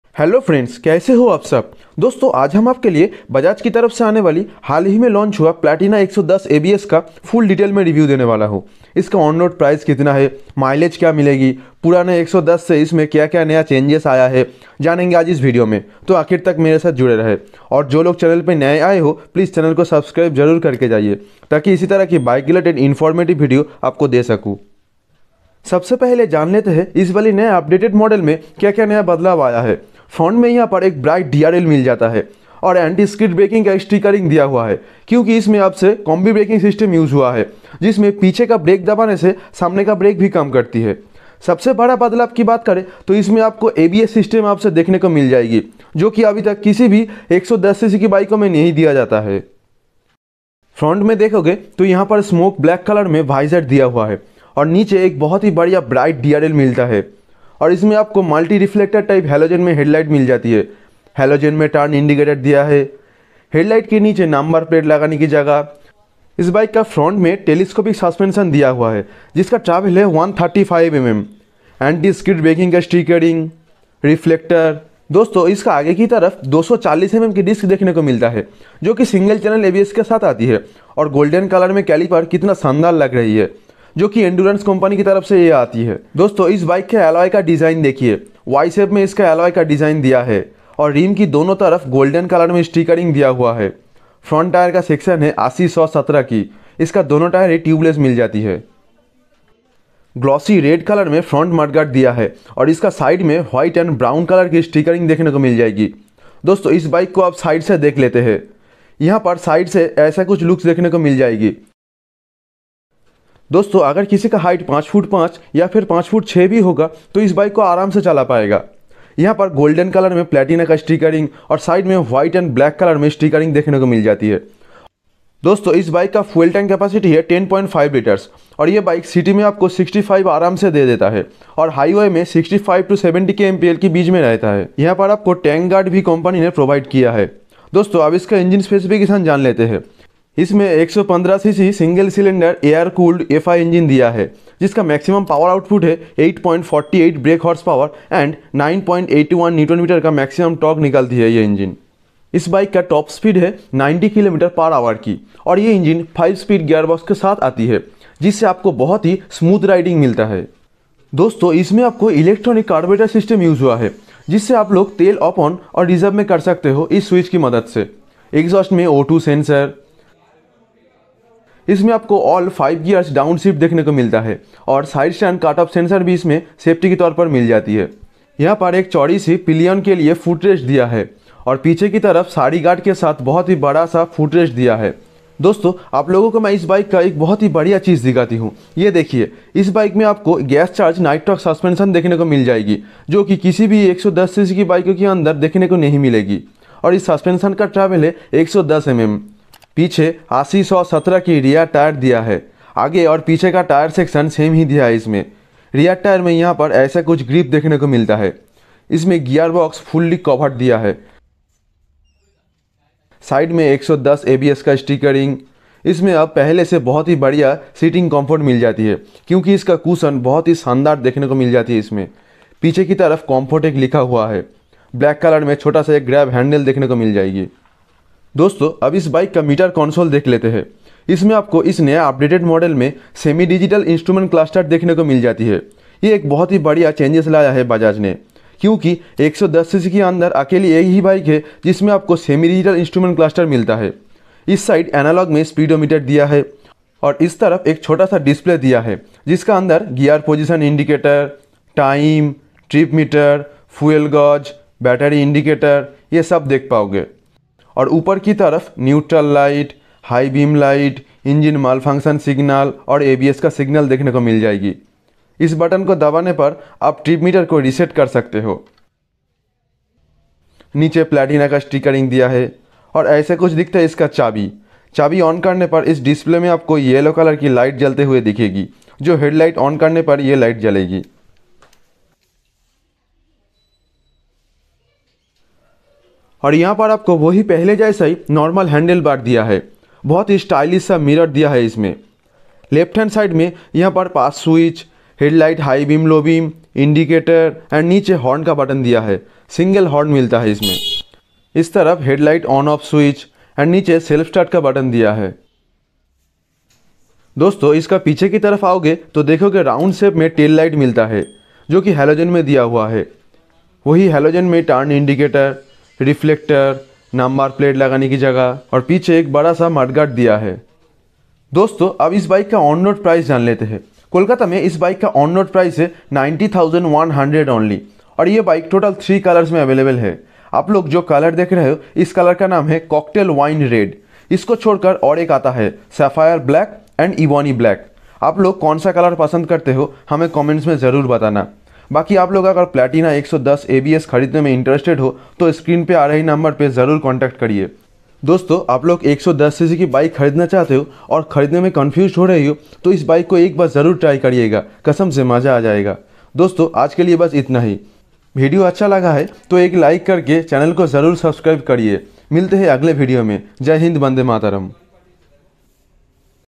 2023 Bajaj Platina ABS - Detailed Review | The Most Safest 110cc Motorcycle | On Road Price, Mileage
2023 Bajaj Platina ABS - Detailed Review | The Most Safest 110cc Motorcycle | On Road Price, Mileage

Doston is video me mene bajaj platina 110 ABS ka review kiya hai jisme mene iske on road price Features Mileage ke bare me bataya hai i hope ye video apko acha lagega so video ko like aur share karen.

VERY SPECIAL THANKS TO OSL BAJAJ MAHESHTALA 🙏

📌SHOWROOM ADDRESS -119, GROUN-FR E5:, 4/60, Gangarampur Rd, Kolkata, West Bengal 700141